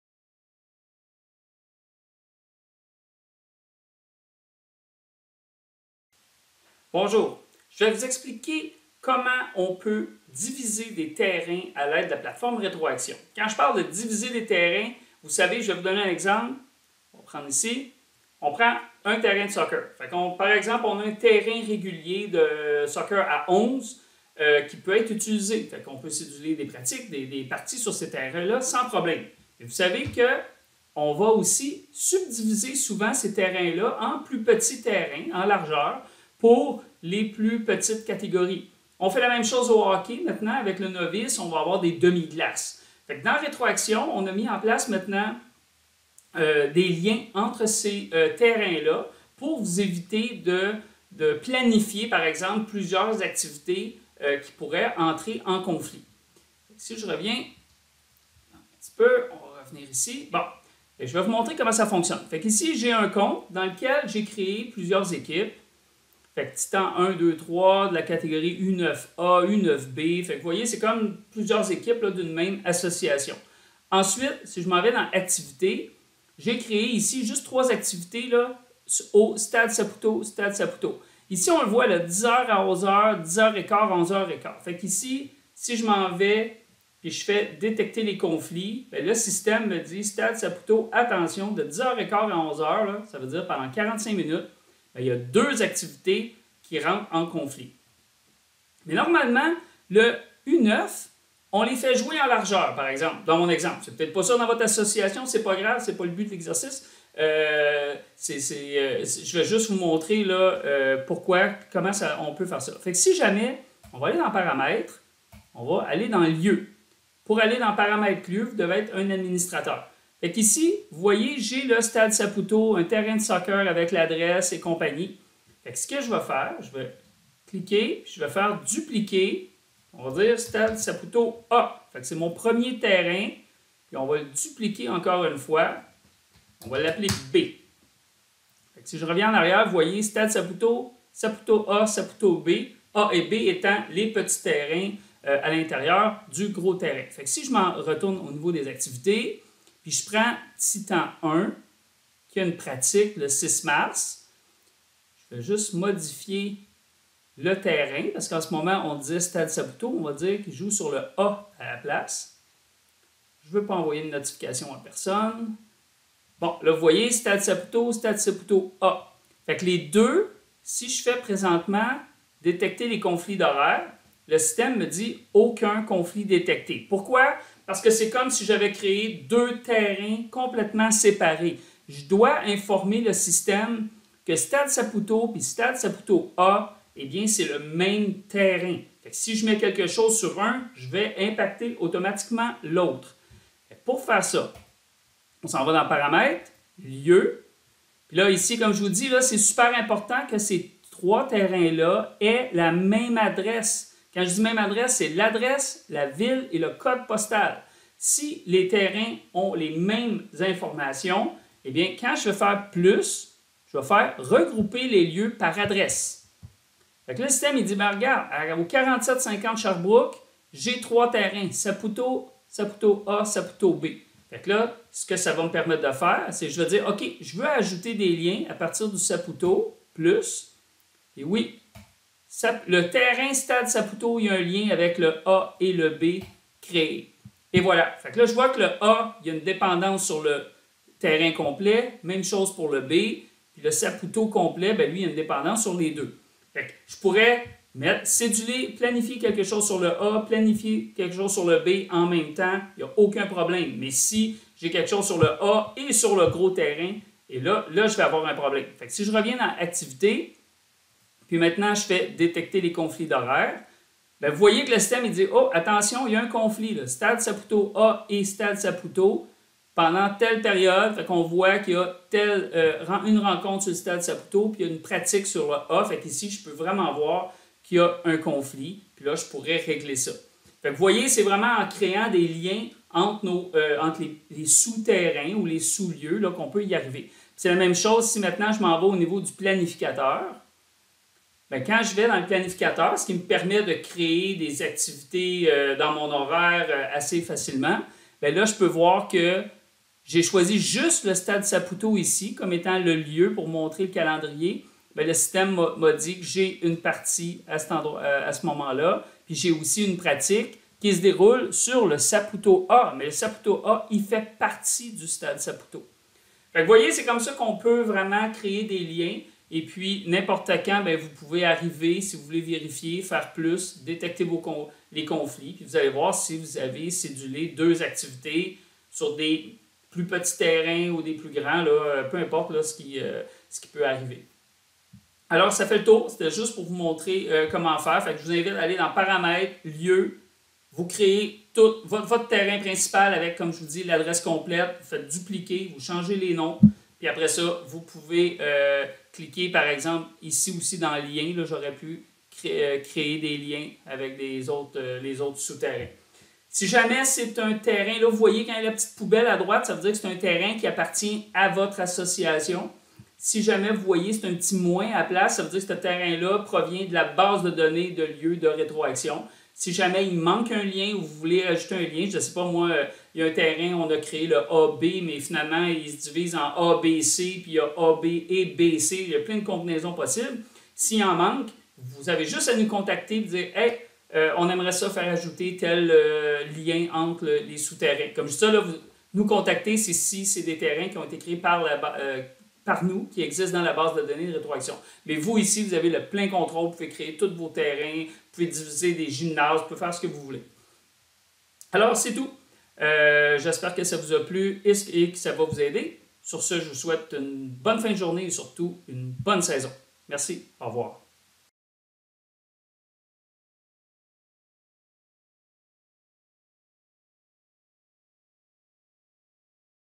Diviser un lieu (terrain, patinoire, gymnase, etc)
Cette vidéo explique comment on peut diviser un lieu (terrain, aréna, gymnase, etc) en plusieurs sous-lieux et détecter s'il existe des conflits lors de notre planification.